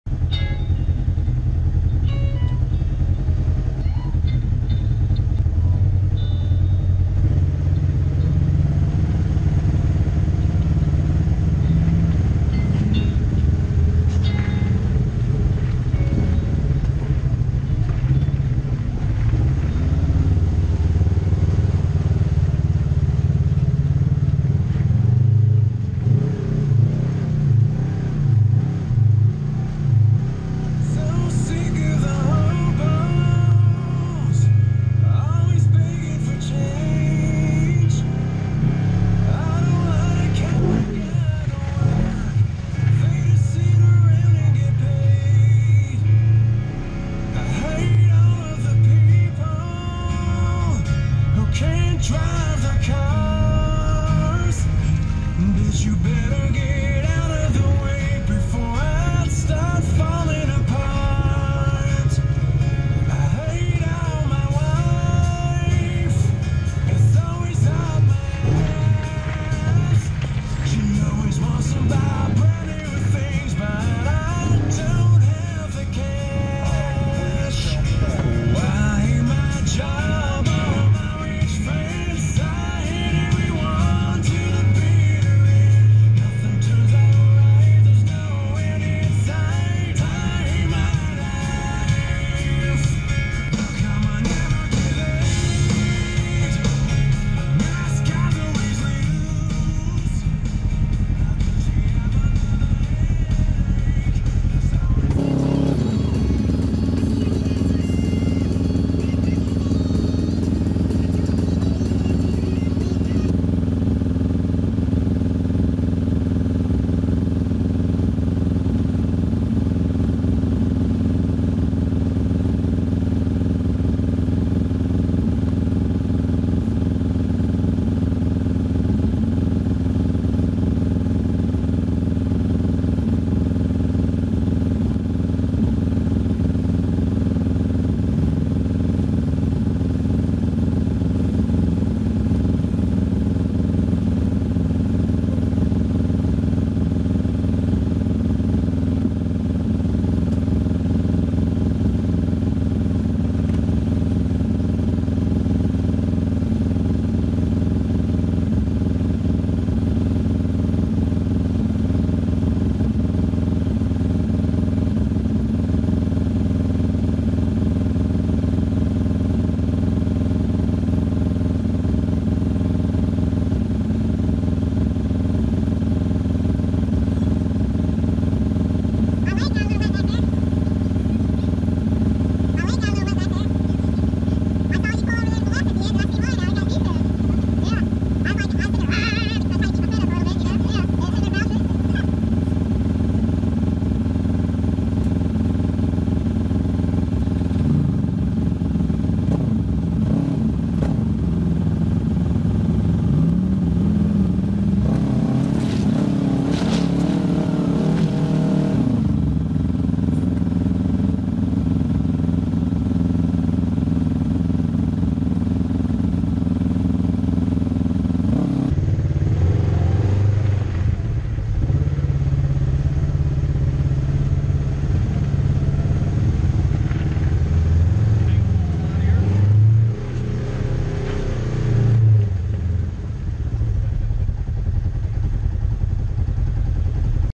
Slade Kentucky - Deep Water | 05/05/2018
A trail that went through some deep water. Had to pull my drain plugs in floor board.